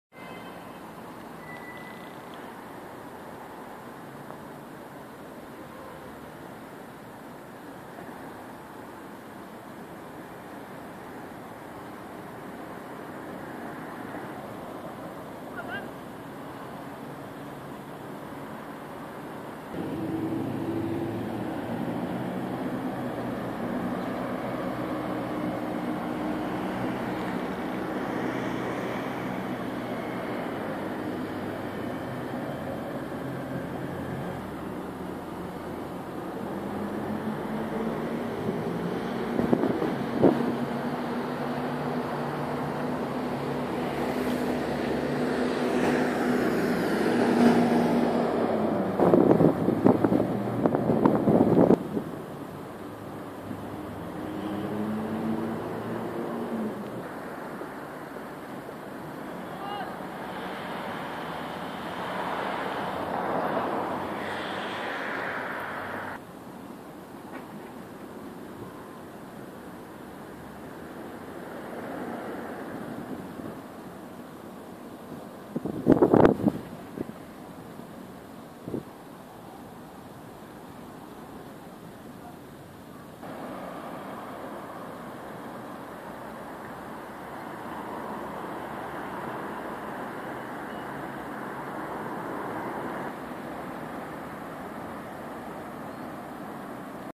Hanley Bus Station remains 16.3.2017. Very Sad :-(
the former PMT bus station in Stoke on Trent, now used as a car park.